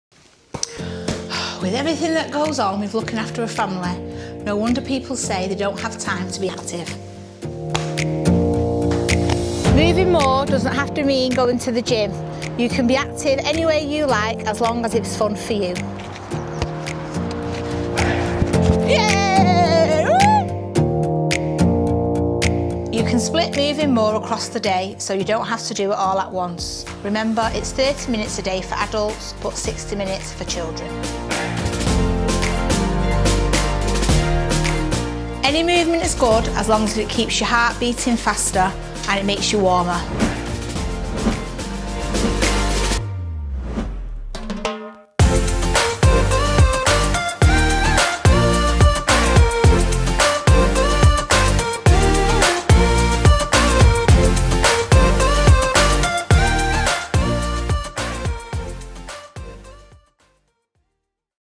Fit families have more fun ThatCounts!
Laura is a busy mum of cheeky young lad James, watch as she tells us how she fits moving more into her day.